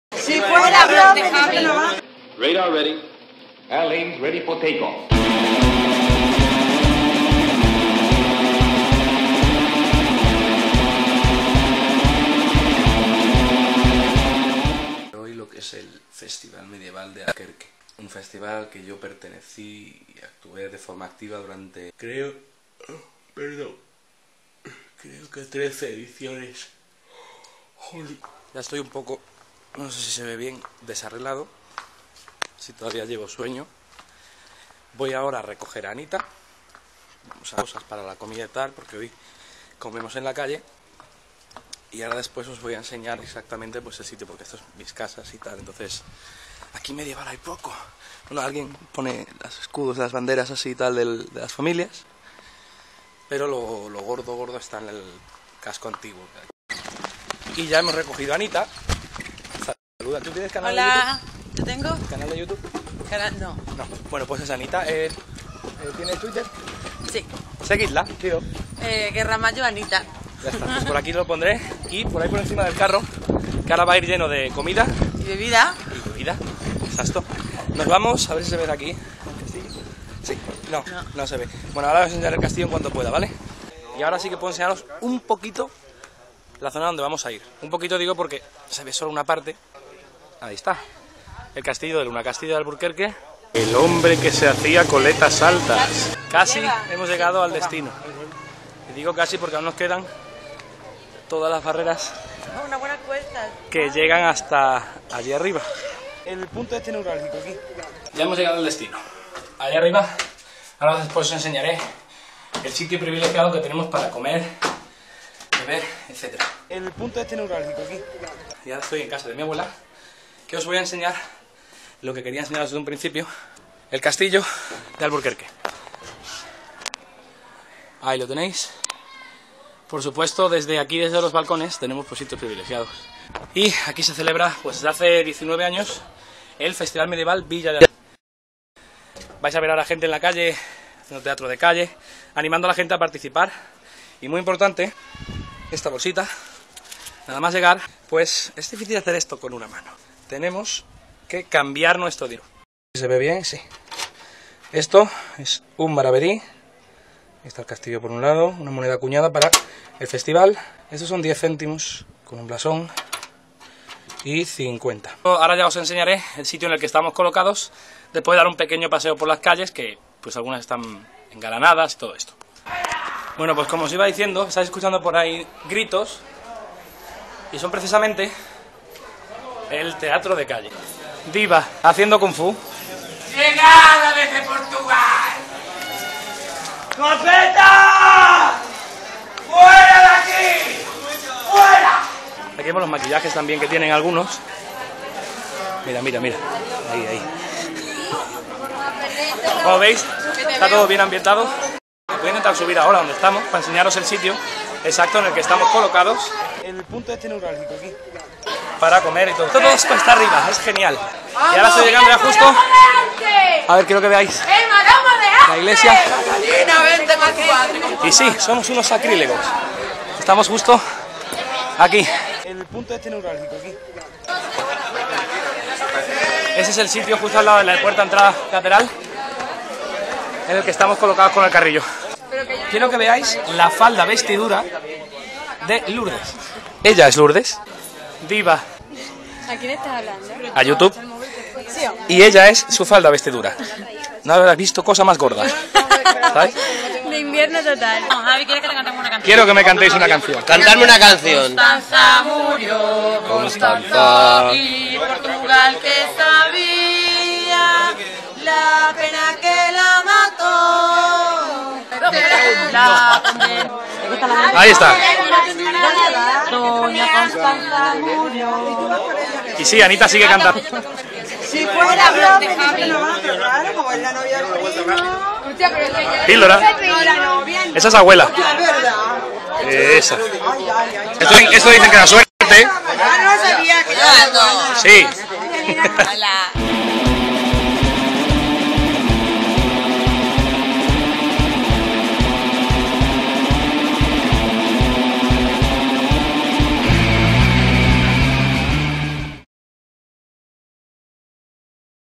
Vlog Medieval
Después de un ataque de algún Hacker (al cual deseo y espero le salgan bultos por todo el cuerpo y sean malignos junto a toda su familia) que me ha borrado todos los videos y suscripciones... I'M BACK

Aquí tenéis lo que pude grabar en el XIX Festival Medieval Villa de Alburquerque... se me olvidó coger el cargador y no puedo enseñaros todo... pero mejor, así vais vosotros mismos a verlo coñe!